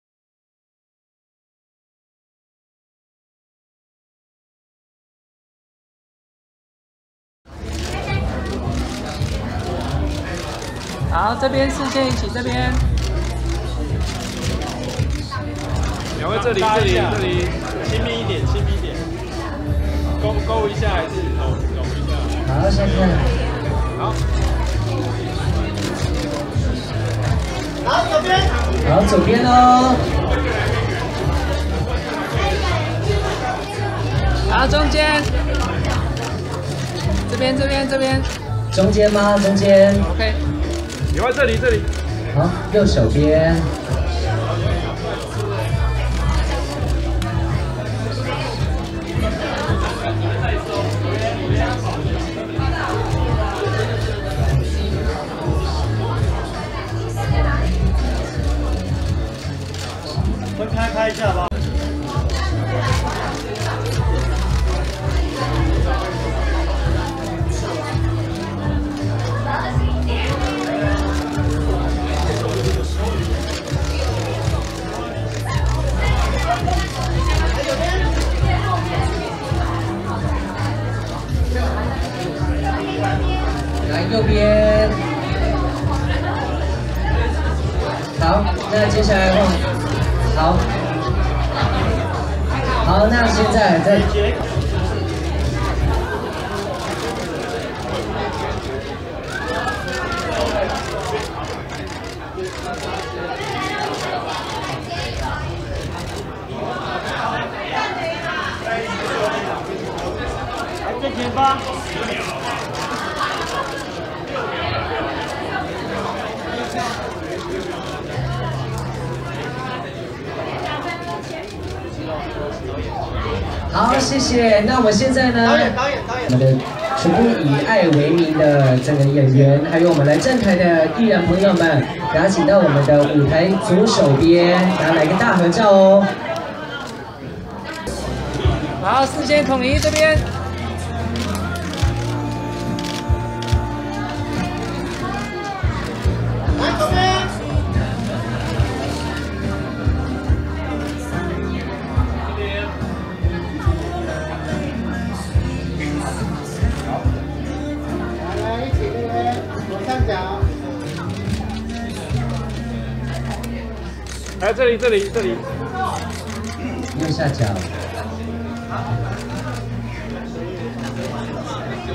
2017-5-17導演王毓雅.男主角黃愷傑.女主角王奕 瑾出席《以愛為名》首映會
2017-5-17導演王毓雅.男主角黃愷傑.女主角王奕
瑾出席《以愛為名》首映會